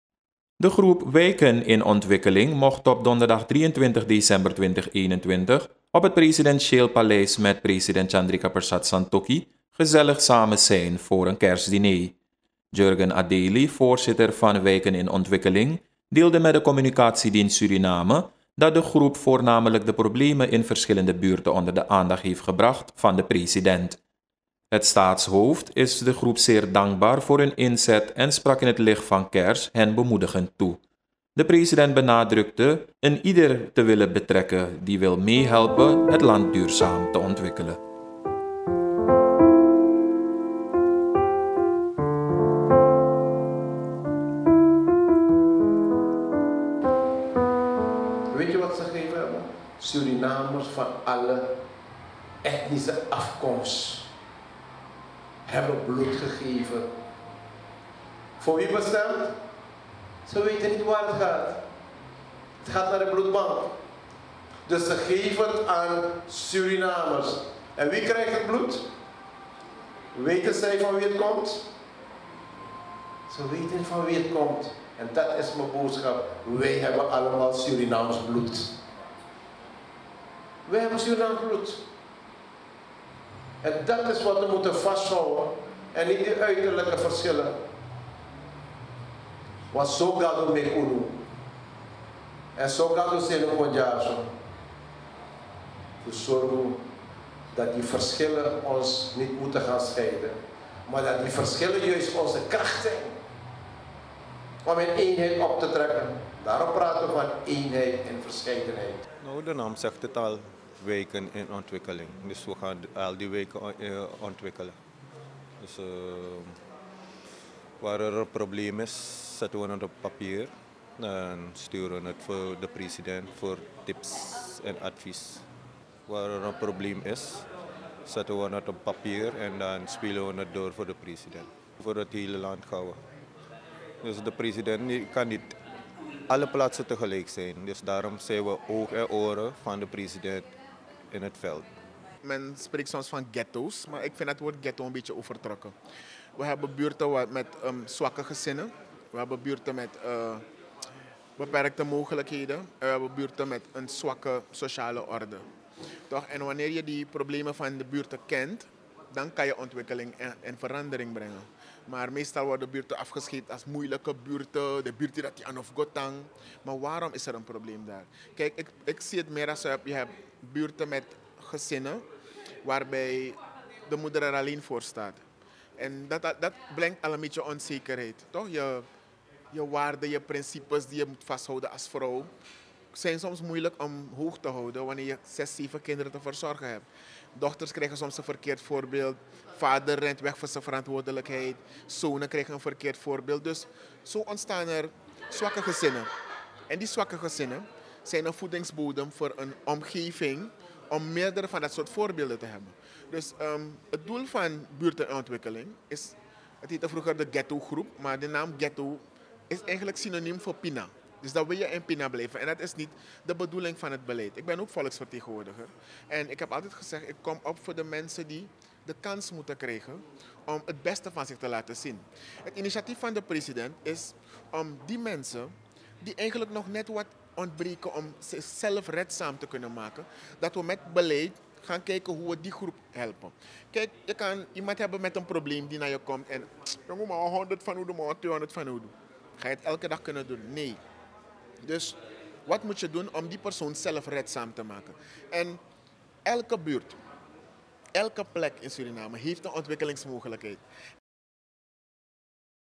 President houdt Kerstdiner met de groep ‘Wijken in Ontwikkeling’
De groep “Wijken in Ontwikkeling” mocht op donderdag 23 december 2021 op het presidentieel paleis met president Chandrikapersad Santokhi gezellig samen zijn voor een kerstdiner. Jurgen Adely, voorzitter van “Wijken in Ontwikkeling” deelde met de Communicatie Dienst Suriname, dat de groep voornamelijk de problemen in verschillende buurten onder de aandacht heeft gebracht van de president. Het staatshoofd is de groep zeer dankbaar voor hun inzet en sprak in het licht van kerstmis hen bemoedigend toe. De president benadrukte iedereen te willen betrekken die willen helpen het land duurzaam te ontwikkelen.